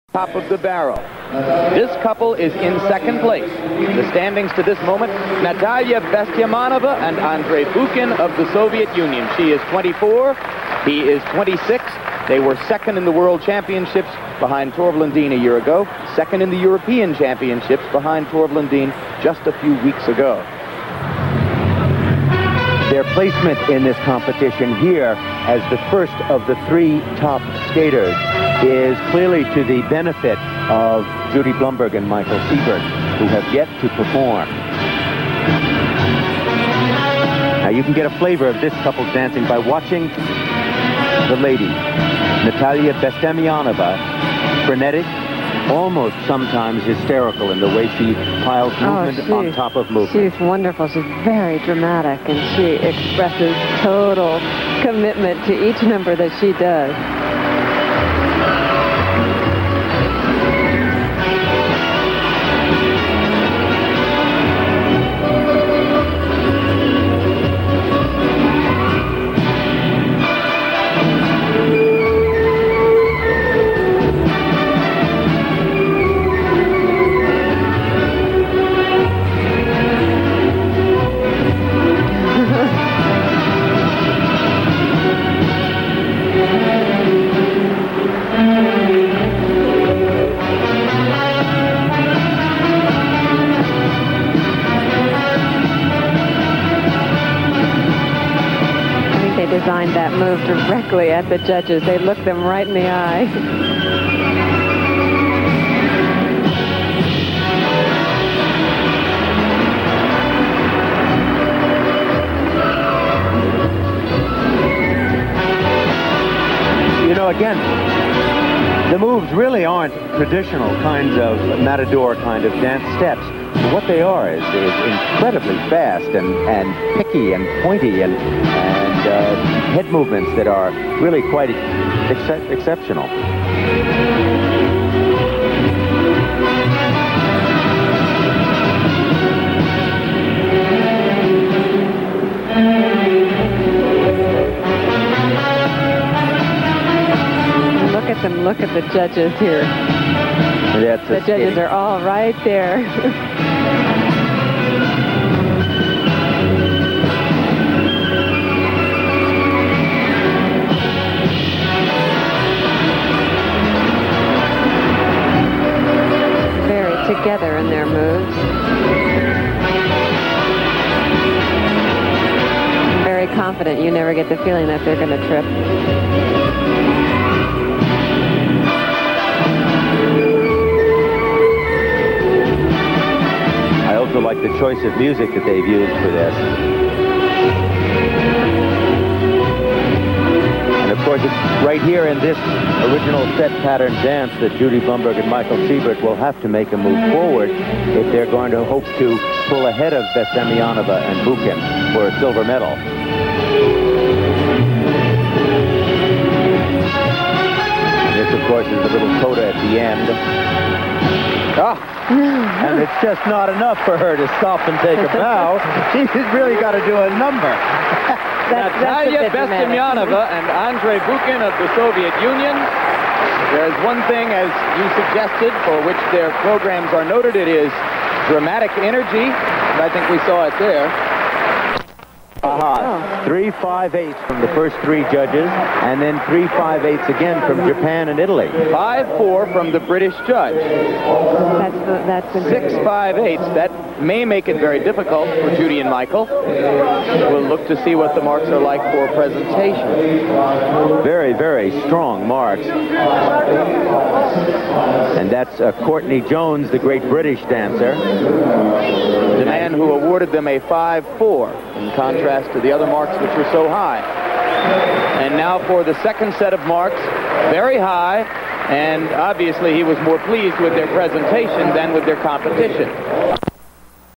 Bestemianova & Bukin (URS) - 1984 Sarajevo, Ice Dancing, Original Set Pattern
Sarajevo, YUGOSLAVIA - 1984 Winter Games, Ice Dancing, Original Set Pattern ("Paso Doble") - Natalia Bestemianova and Andrei Bukin of the Soviet Union remained in 2nd Place after this dance.